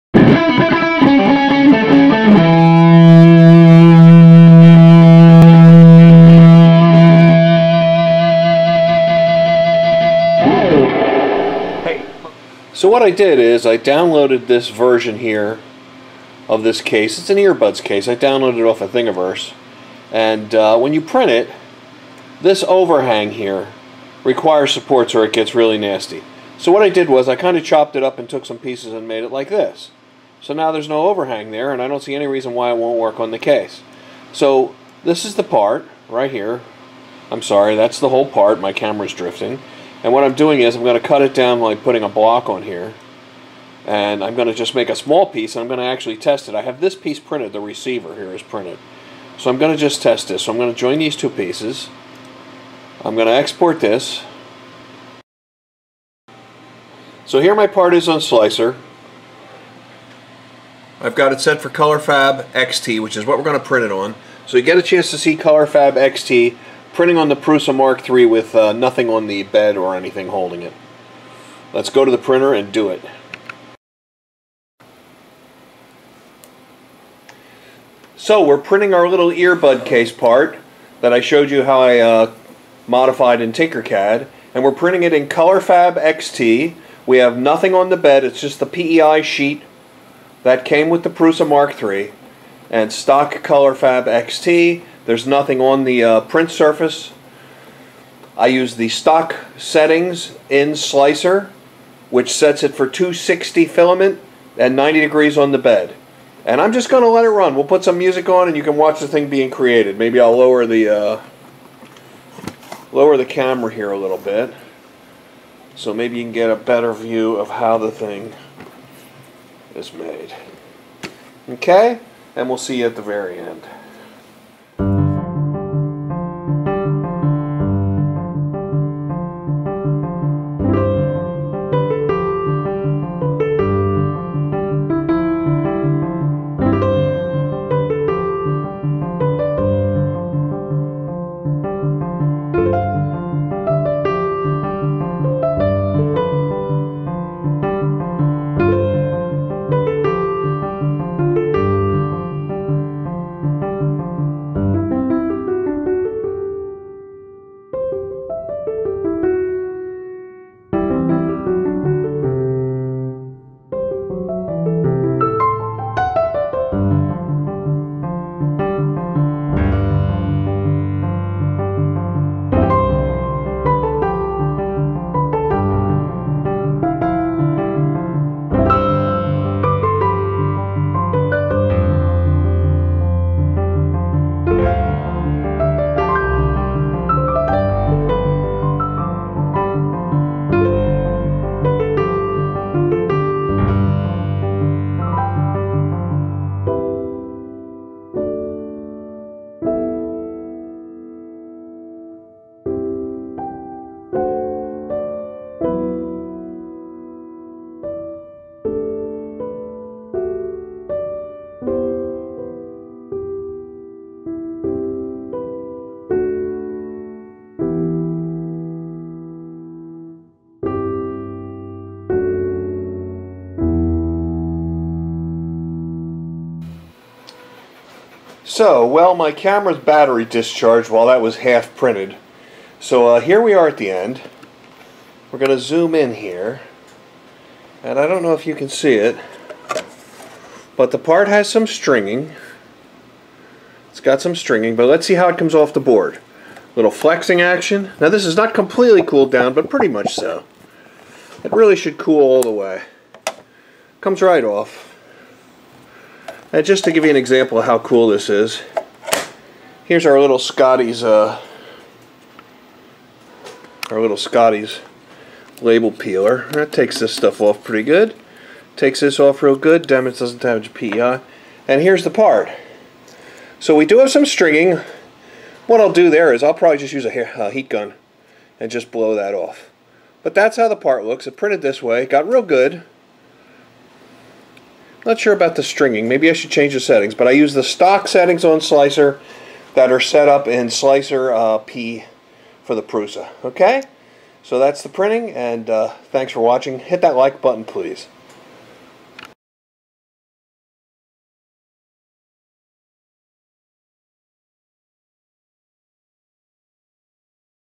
COLORFABB XT ON PRUSA MK3 AND TINKERCAD QUICK DEMO
Just a very quick demo of a part I'm modifying in Tinkercad and printing the test piece on the Prusa i3 Mk3 using Colorfabb XT Filament.  Hit "SHOW MORE" to scroll down and see more useful links etc.